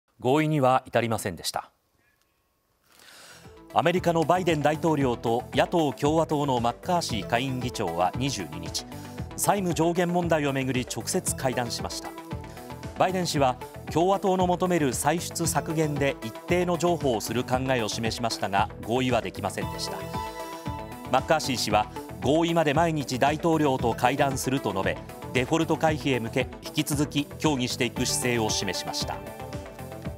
債務上限問題 バイデン氏とマッカーシー氏会談も合意には至らず（2023年5月23日）
合意にはいたりませんでした。アメリカのバイデン大統領と野党共和党のマッカーシー下院議長は22日、債務上限問題を巡り直接、会談しました。バイデン氏は共和党の求める歳出削減で一定の譲歩をする考えを示しましたが、合意はできませんでした。マッカーシー氏は合意まで毎日、大統領と会談すると述べ、デフォルト回避へ向け引き続き協議していく姿勢を示しました。